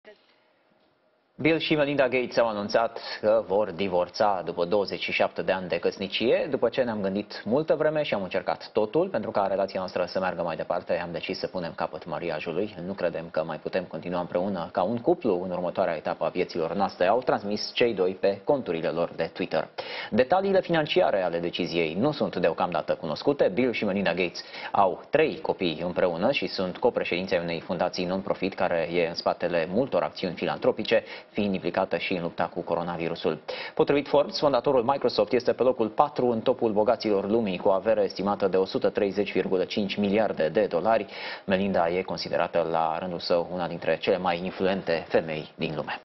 Bill Gates divorţează de soţia sa, Melinda, după 27 de ani de căsnicie
Bill şi Melinda Gates au anunţat că vor divorţa, după 27 de ani de căsnicie. Cei doi vor continua să colaboreze în cadrul Fundaţiei Bill şi Melinda Gates.